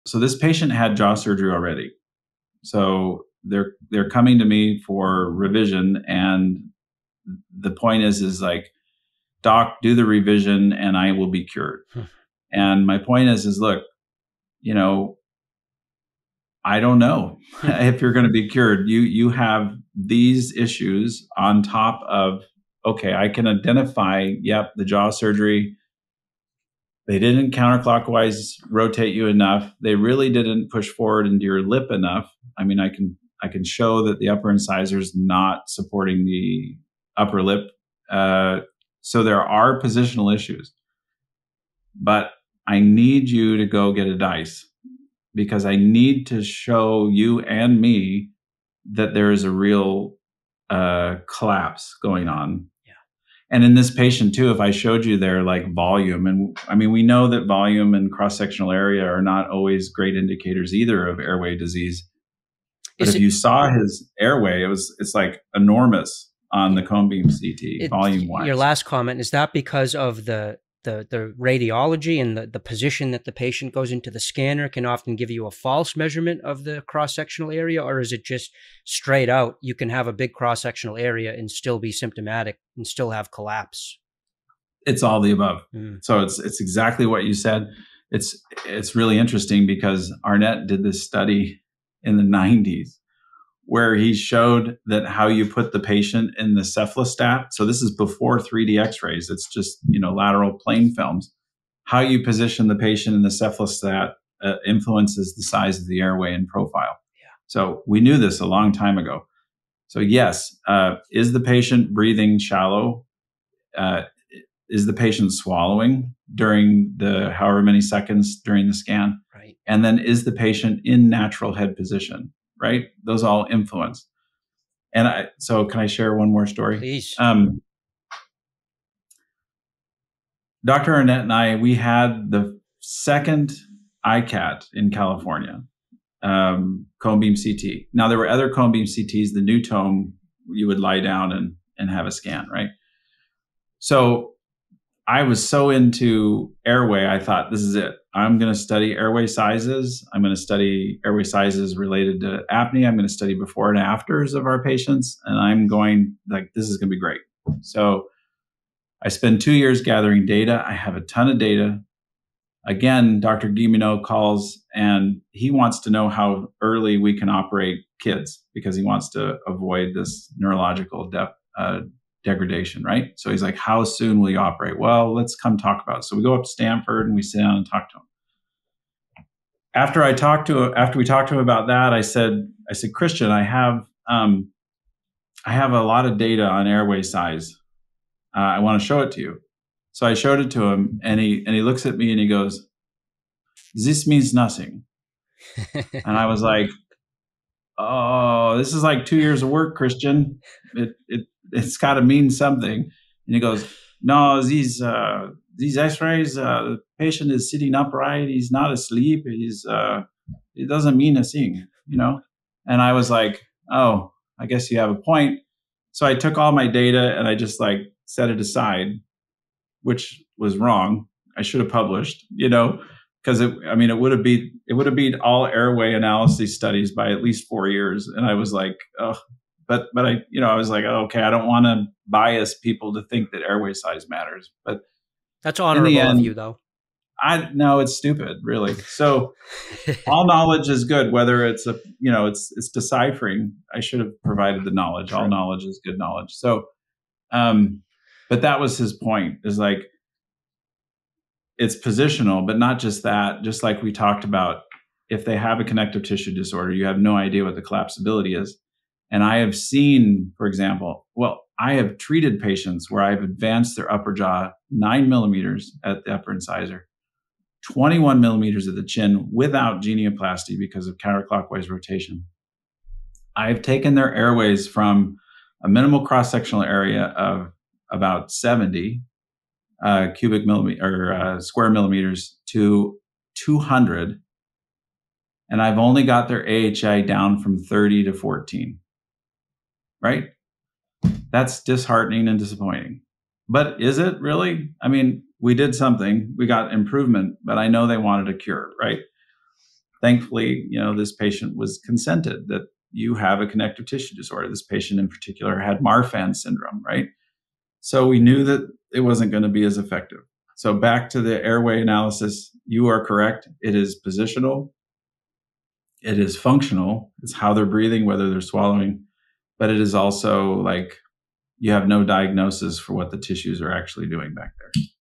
Why Airway Analysis Doesn't Tell the Full Story
// Oral surgeon Dr. Michael Gunson explores the complexities and limitations of airway analysis in orthognathic surgery, examining why static airway measurements from CT scans may not always correlate with clinical outcomes.

Timestamps
00:00 - Revision jaw surgery case intro
01:10 - Imaging limitations in airway analysis
02:35 - Stanford consultation story about airway data
04:10 - Factors influencing airway collapse and support